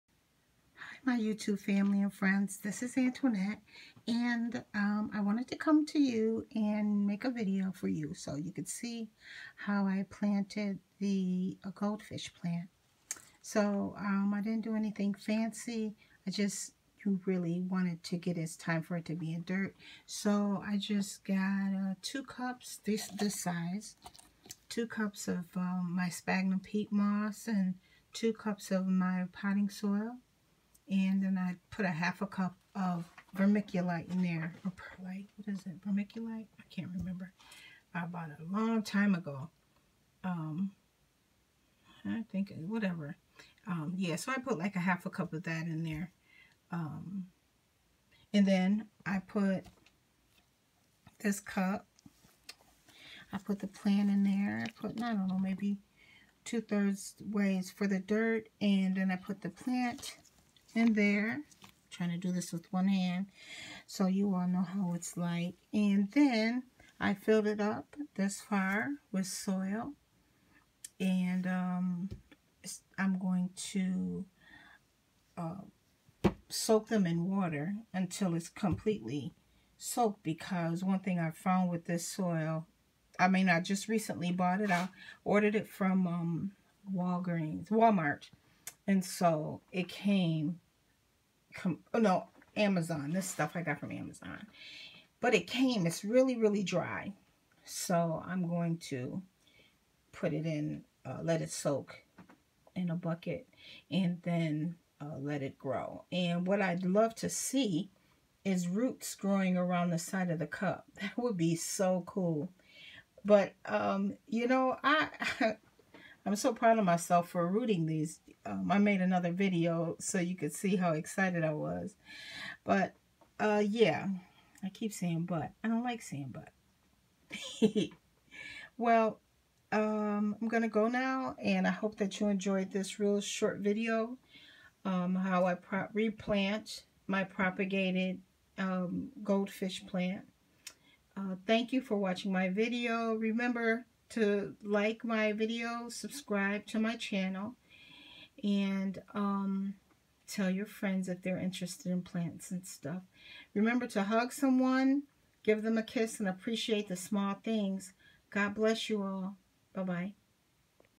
Planting my propagated “Gold Fish” Cuttings - Columnea Gloriosa
Time to plant 🌱 our gold fish cuttings. I used potting soil and sphagnum moss. Don’t forget to cut a small hole in cup for drainage and water til completely wet. Place in an indirect window and watch it grow. It gets beautiful little flowers that looks like bright orange gold fish. Good luck!